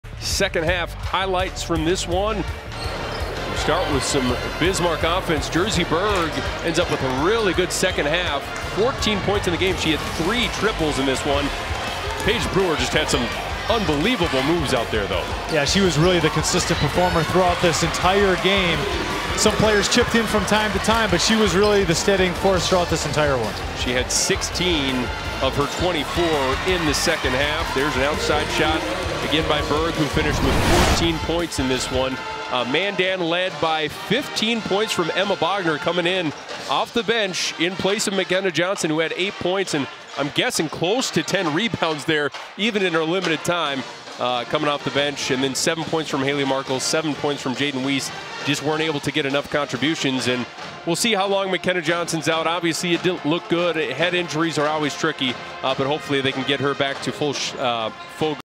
Bismarck v.s. Mandan (62-46) | 12/6/22
Bismarck Demons defeats Mandan Braves at Mandan High School in Mandan, ND. Watch the second half highlights here.